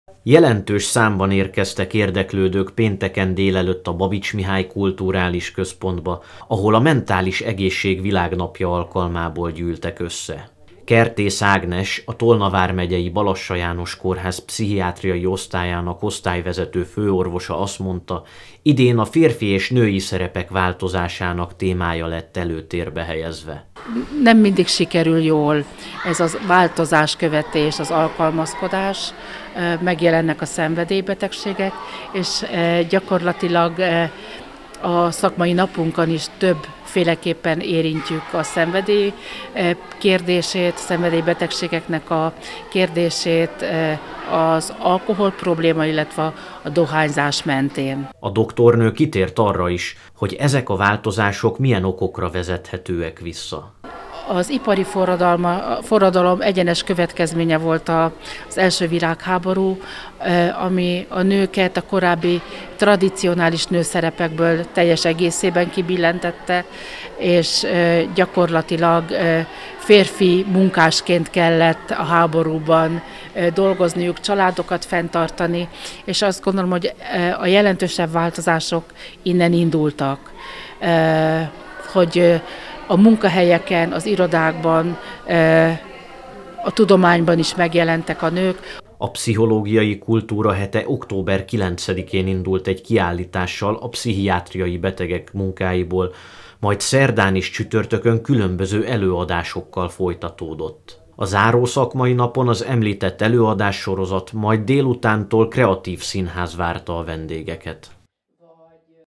Női és férfi szerepek változásai - Előadás-sorozat a Babits Mihály Kulturális Központban
Október tizedike a Mentális egészség Világnapja. Ennek alkalmából pénteken délelőtt különböző előadások várták az érdeklődőket Szekszárdon a Babits Mihály Kulturális Központban. Ezzel együtt a Tolna Vármegyei Balassa János Kórház Pszichiátriai Osztálya nyílt napot is tartott, ahol a látogatók betekintést nyerhettek az osztály mindennapi életébe, terápiás munkájába.